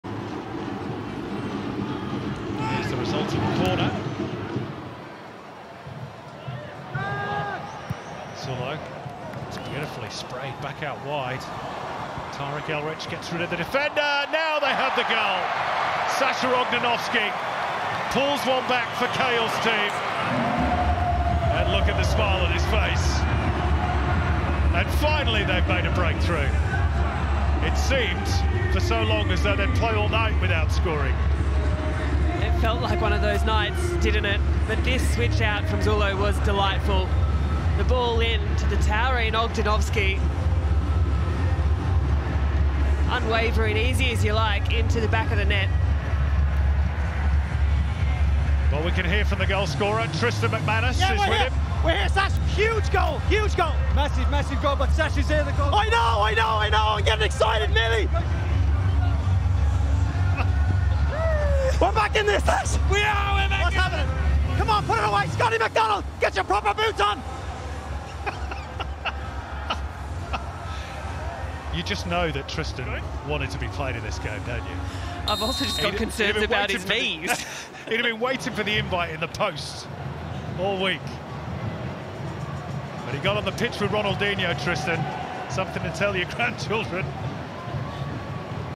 OGNENOVSKI SCORES FOR THE CAHILLXI
OGNENOVSKI!!
Here's how the CahillXI got onto the scoreboard vs the RonaldinhoXI... and how Tristan Macmanus got onto the field? ⚽🔥😲